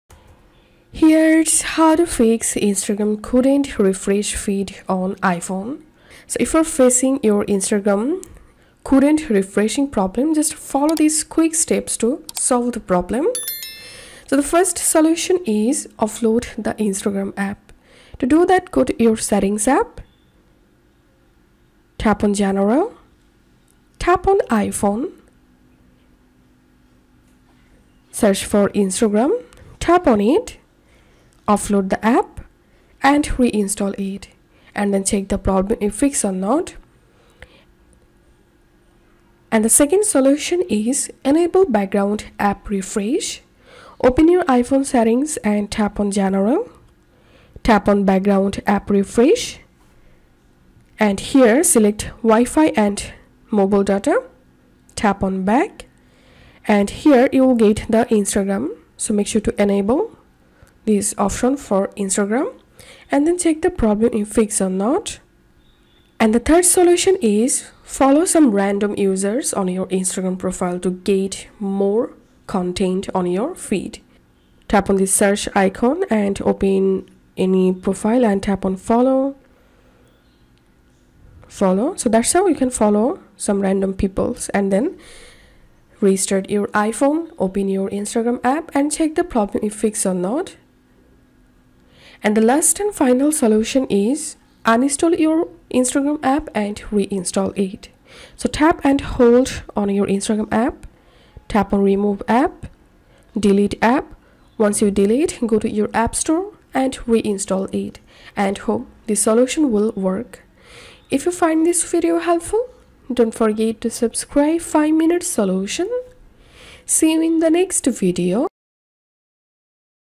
Fix Instagram Couldn't Refresh Feed on iPhone or iPad 2024 | 100% Solved Problems
Instagram couldn't refresh the feed? Why does this error occur? How to fix the Instagram feed not updating error? Try the following methods in the tutorial.

0:00 Fix Instagram couldn't refresh the iPhone
0:17 Update Instagram on iPhone
0:40 Enable background app refresh iPhone
1:04 Follow someone on Instagram
1:32 Reinstall Instagram on iphone
1:52 Outro